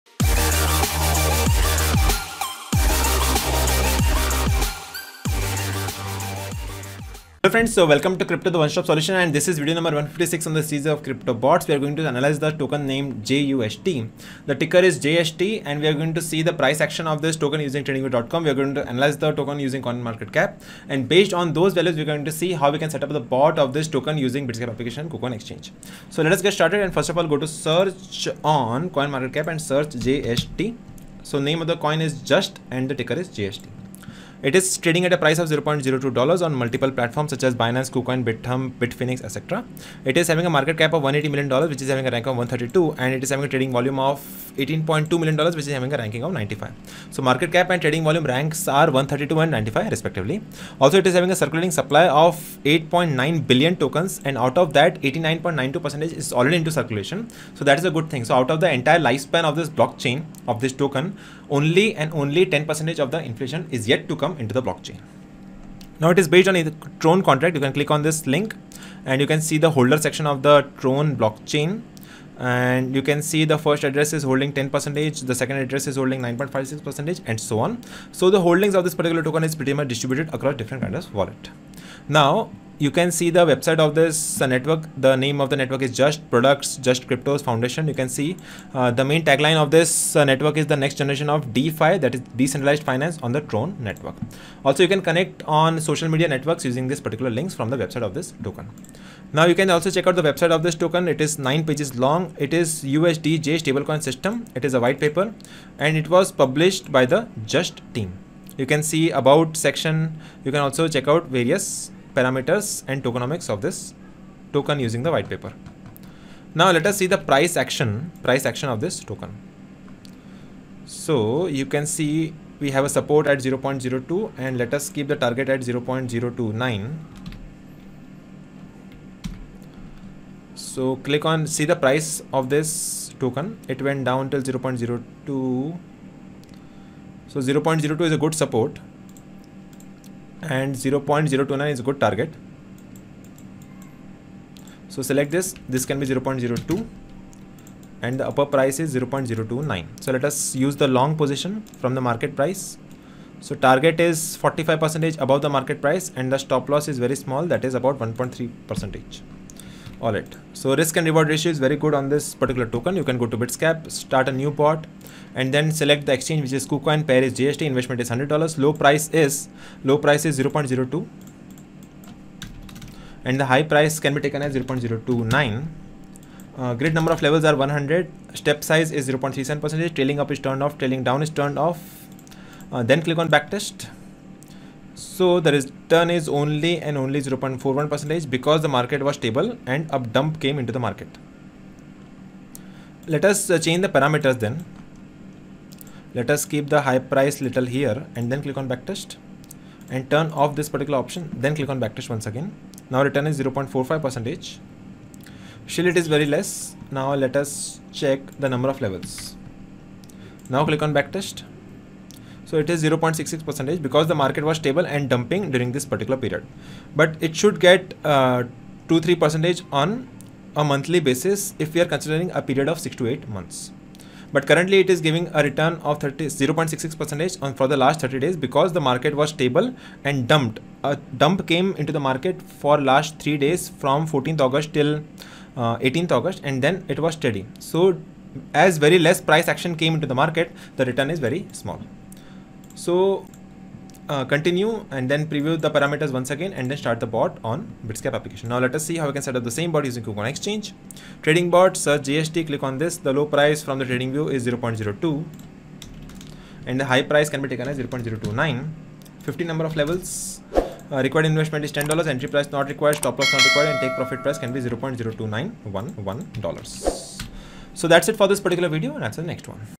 #156 | JUST | Crypto Bots | Spot Grid | Kucoin & Bits Gap
Hello Friends,

This is a video Series on Crypto Currency Automation.

We have covered Spot Grid Automation and Shortlisted coins list analysis.

Kucoin and Bits Gap will be covered including all parameters and analysis details.

Sign Up to join Kucoin Exchange:-